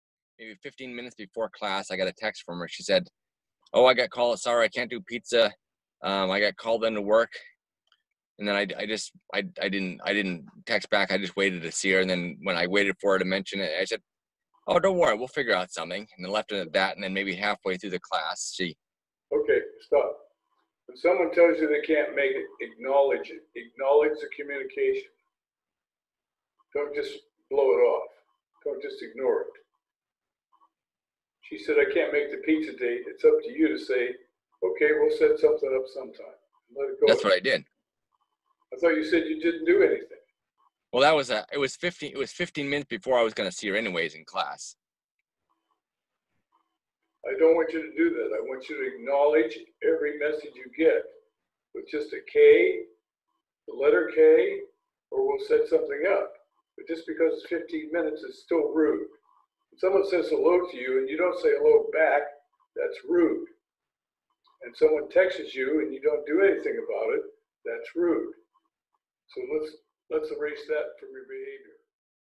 TEXTING ETIQUETTE
Someone says "Hello" you say Hi or Howdy. Same with text. Sometimes we wait for hours but we always RESPOND even with K. It's polite.
ALL THE ANSWERS are here: DATE YOUNG WOMEN 21ST CENTURY